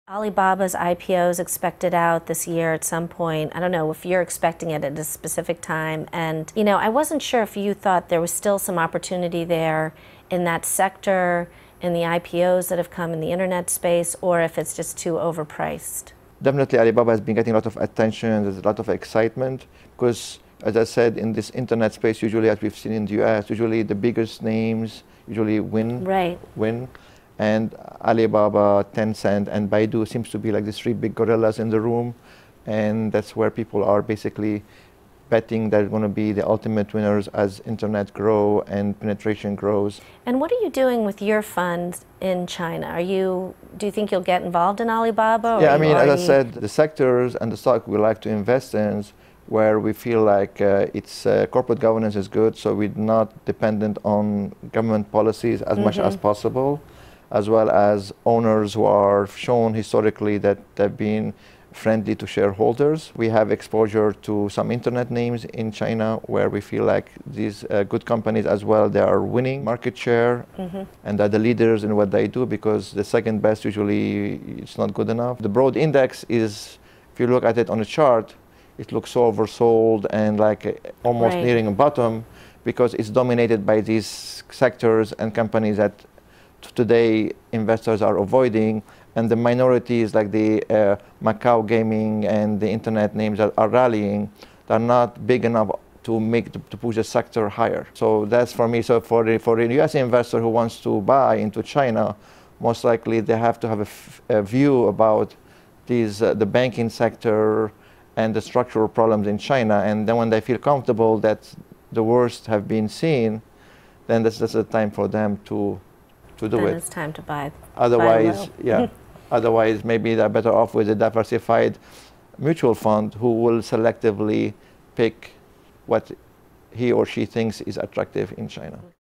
The risks of investing in China
The Chinese e-commerce giant Alibaba has received a lot of attention, but there are risks. Riad Younes, co-portfolio manager of the RSQ International Equity Fund, discusses his plan for investing in China.